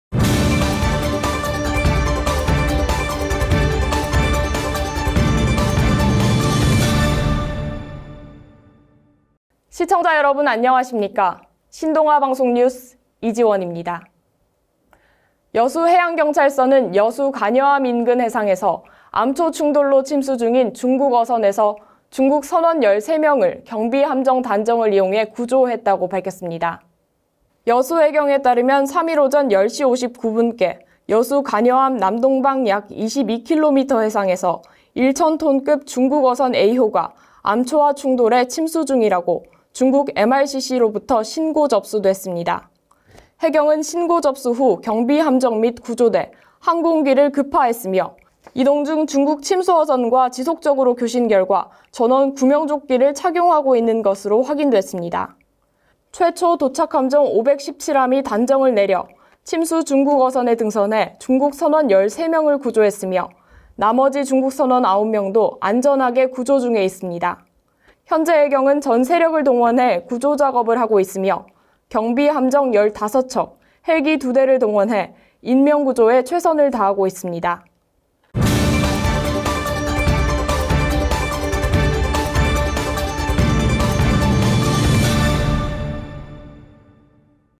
여수해경, 중국어선 암초 충돌로 침수... 구조 중 /SDATV 신동아방송뉴스
여수해양경찰서는 “여수 간여암 인근 해상에서 암초 충돌로 침수 중인 중국어선에서 중국선원 13명을 경비함정 단정을 이용해 구조했다”고 밝혔습니다. 여수해경에 따르면 3일 오전 10시 59분께 여수 간여암 남동방 약22km해상에서 1천톤급 중국어선 A호(승선원 22명)이 암초와 충돌해 침수 중이다고 중국 MRCC (해상구조본부)로 부터 신고 접수됐습니다. 해경은 신고 접수 후 경비함정 및 구조대, 항공기를 급파했으며, 이동중 중국 침수어선과 지속적으로 교신결과 전원 구명조끼를 착용하고 있는 것으로 확인됐습니다. 최초 도착함정 517함이 단정을 내려 침수 중국어선에 등선하여 중국선원 13명 구조하였으며, 나머지 중국선원 9명도 안전하게 구조중에 있습니다. 현재 해경은 전 세력을 동원하여 구조작업을 하고 있으며, 경비함정 15척, 헬기 2대를 동원 인명구조에 최선을 다하고 있습니다.